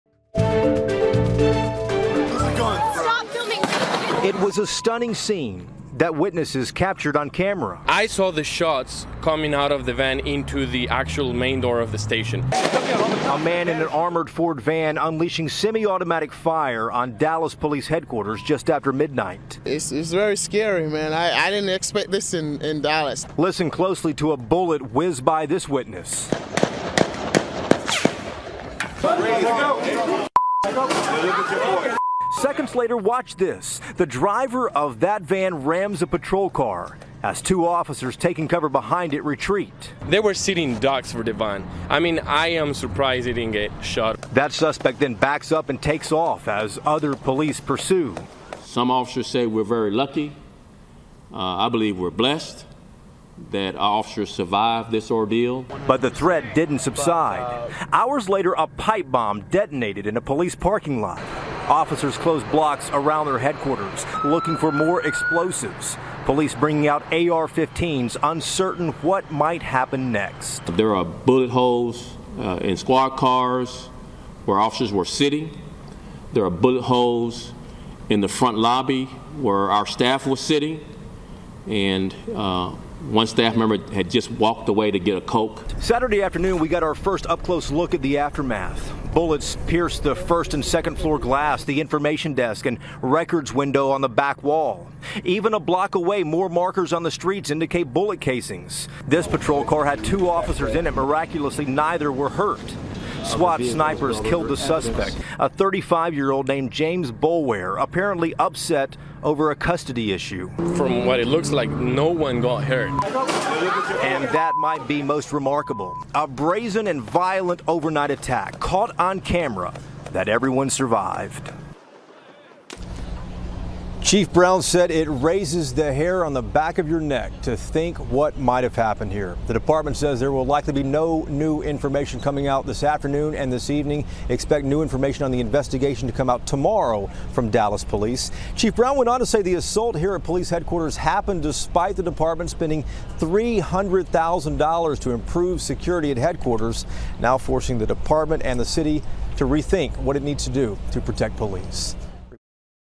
Video captures attack on Dallas police headquarters
Dallas police have confirmed that the suspected lone gunman involved in a shooting at Dallas police headquarters has been killed. Cell phone video captured the chaotic scene in the early morning hours on Saturday.